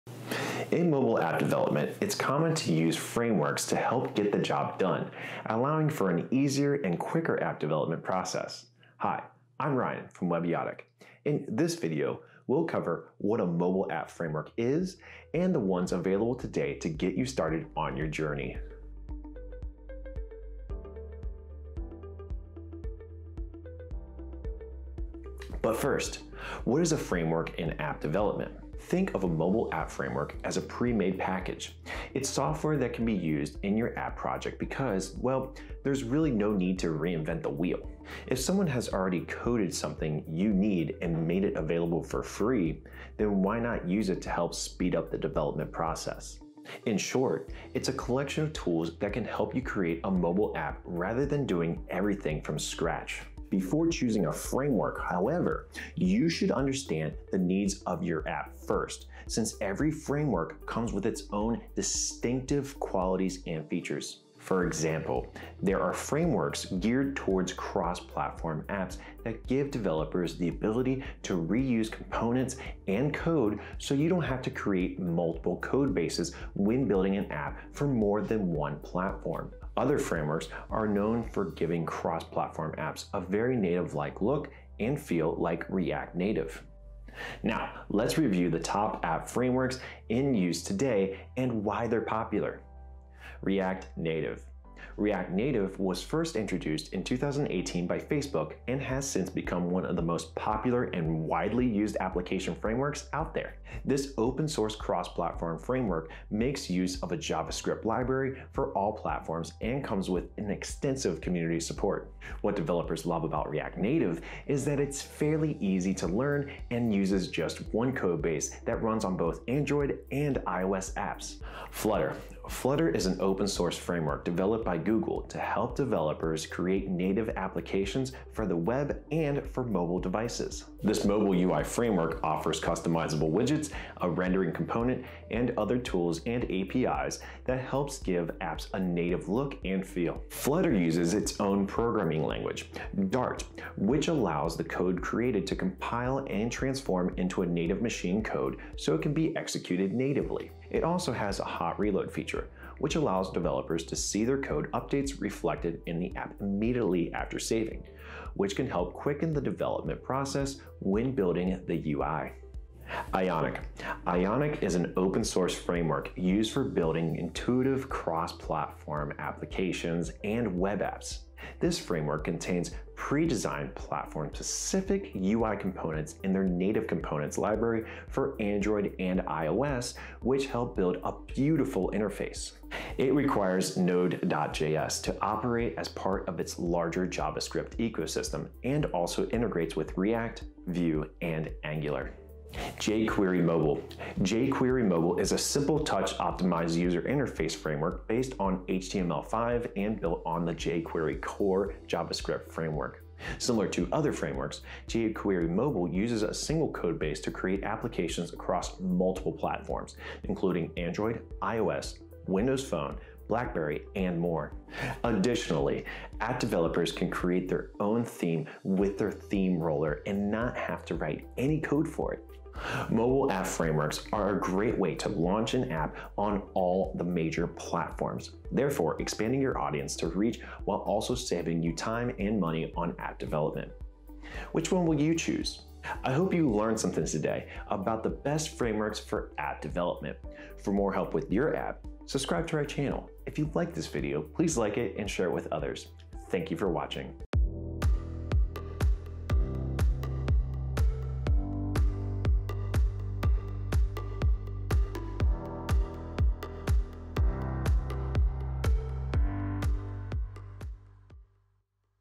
Best Frameworks For Mobile App Development
What are the best frameworks for mobile app development?

Think of a mobile app framework as a pre-made package. It’s software that can be used in your app project because, well, there’s really no need to reinvent the wheel.

If someone has already coded something you need and made it available for free (open-source), then why not use it to help speed up the development process?

Frameworks allow app developers to write large applications much faster and with fewer issues as well as use features in a very standardized way.

In short, it’s a collection of tools that can help you create a mobile app rather than doing everything from scratch.

Every developer should make use of frameworks, even if it's a minimal framework just for user interface (UI) attractiveness.

Before choosing a framework, you should understand the needs of your app first, since every framework comes with its own distinctive qualities and features.

For example, there are frameworks geared towards cross-platform apps that give developers the ability to reuse components and code so you don’t have to create multiple codebases when building an app for more than one platform.

Other frameworks are known for giving cross platform apps a very native-like look and feel, like React Native.

In app development, it’s common to use frameworks to help get the job done, allowing for an easier and quicker app development process.

In this video, we’ll cover what a mobile app framework is and the best app frameworks available today to get you started on your journey.